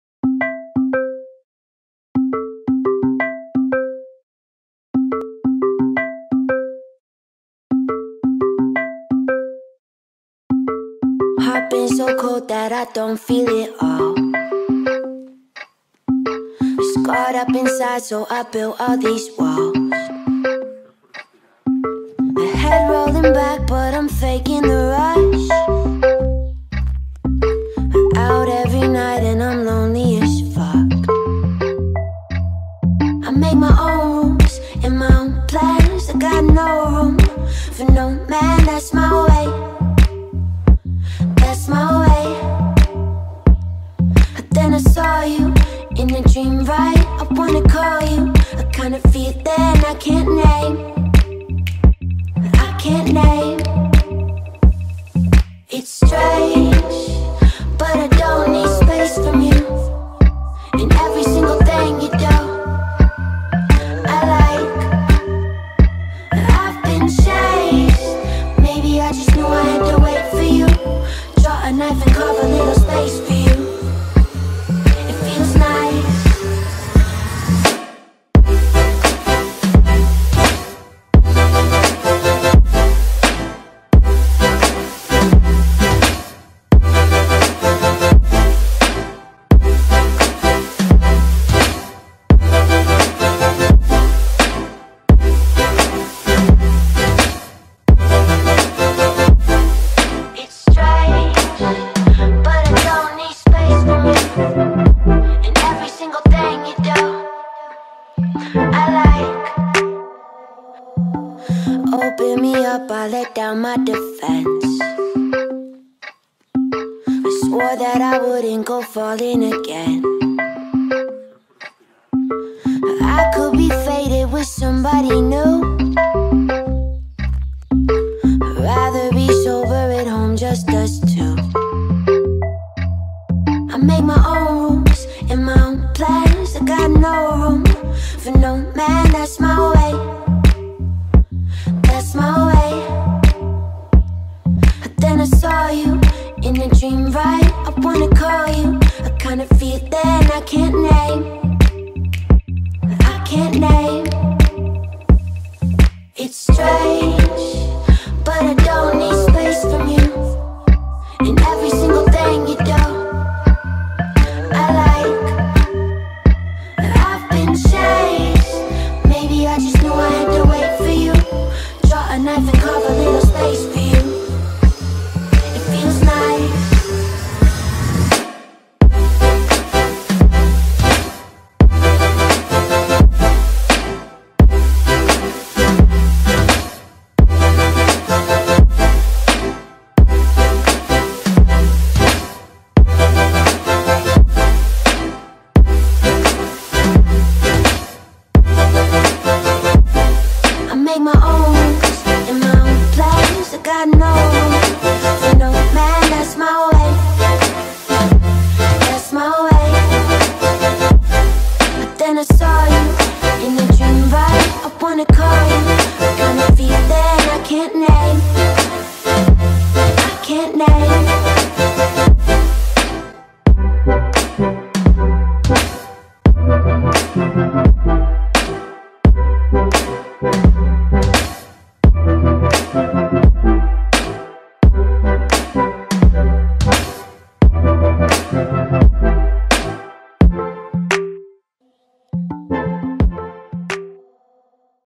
Epic Drone flight in Eifel (Germany) | DJI Phantom 3 Standard
🢡 All shots have been taken with the DJI Phantom 3 Standard